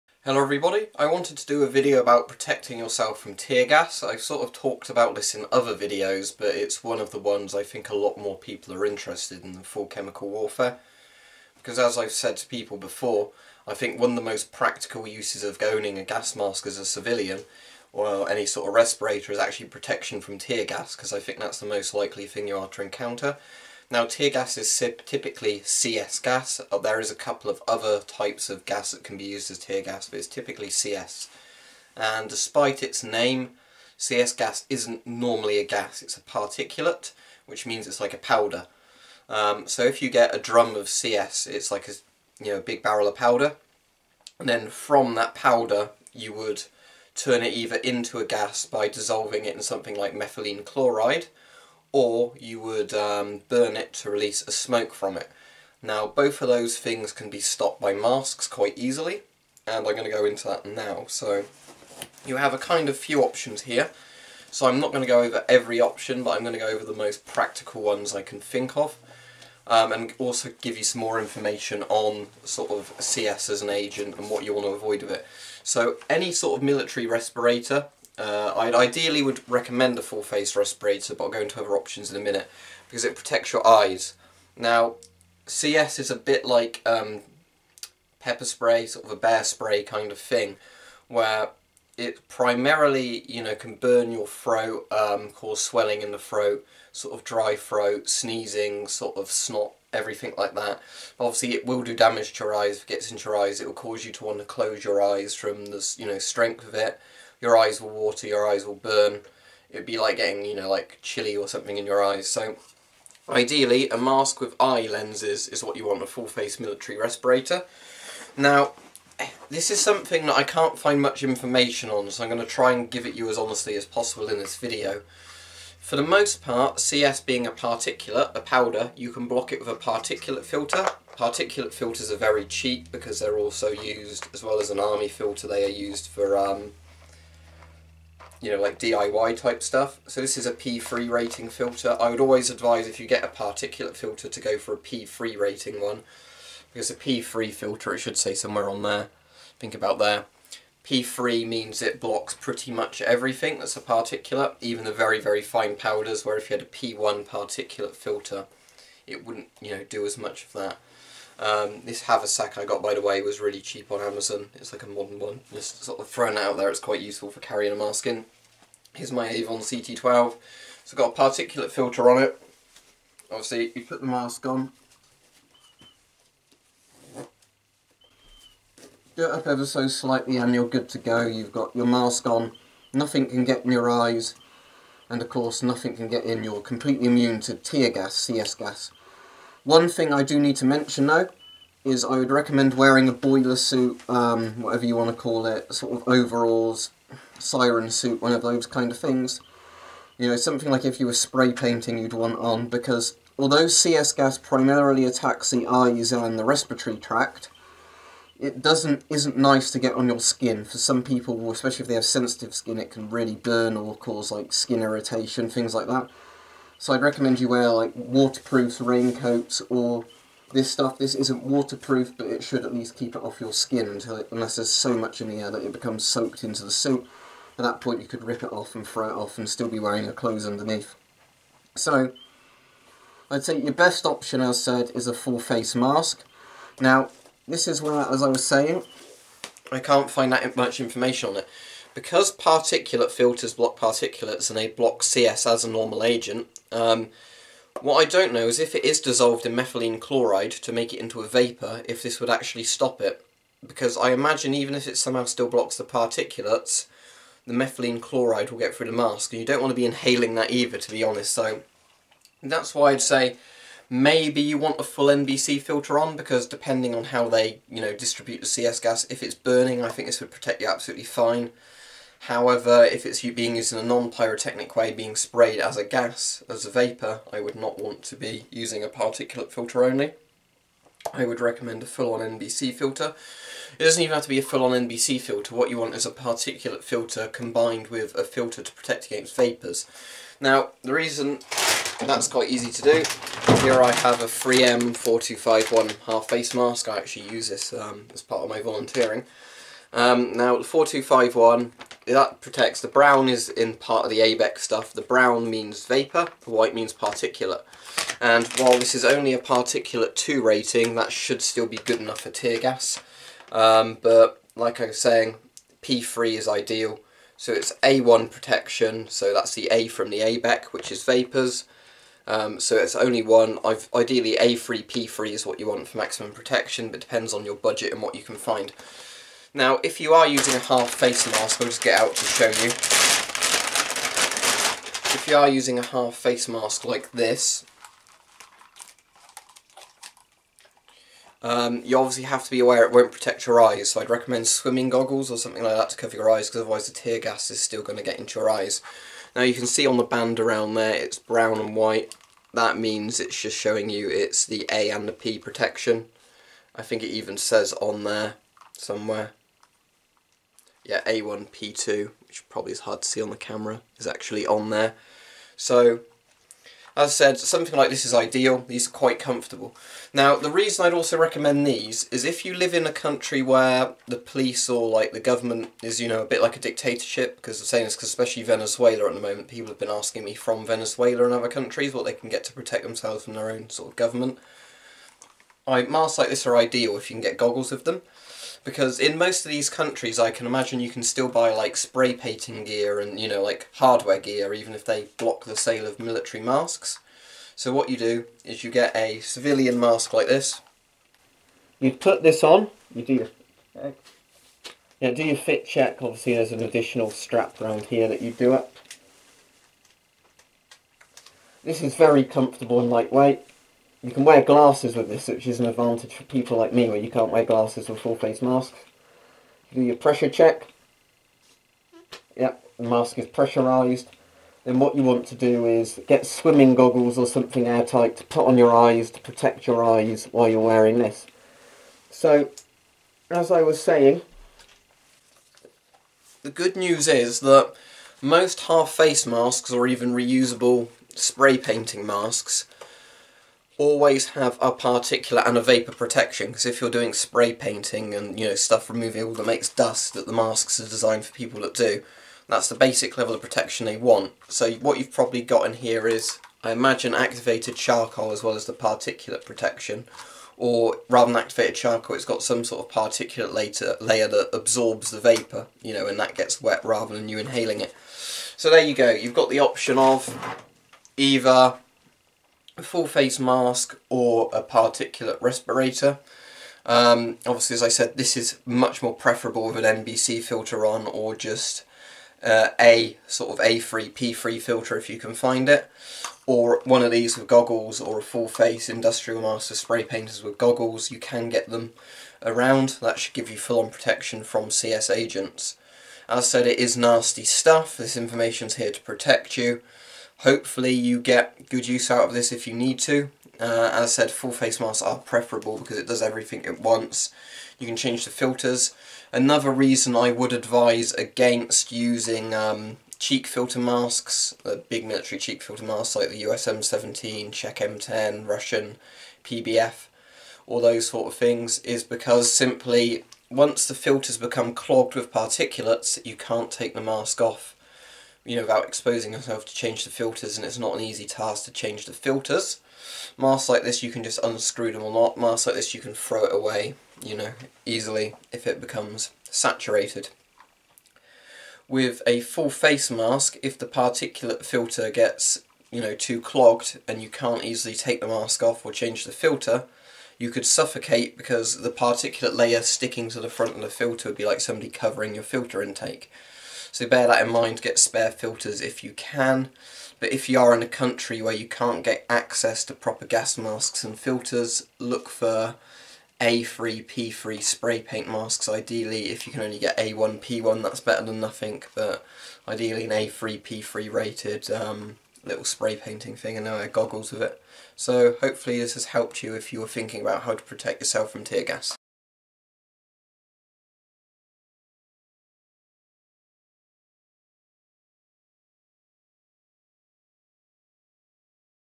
How to protect yourself from Tear Gas
I get asked about this a lot and do other videos that touch on it, so this is a video about how to properly protect yourself from tear gas.
Tear gas, technically CS powder, is a chemical powder that causes skin and eye irritation, the powder can be dissolved in methylene chloride and distributed as a vapour, or can be burned to be released as a powder.
Tear gas primarily attacks the respiratory system as well as the eyes, secondarily it causes skin irritation and pain.
For full protection, you will need a mask that offers particulate and vapour protection, as well as a full body suit such as a boiler suit or a dust suit.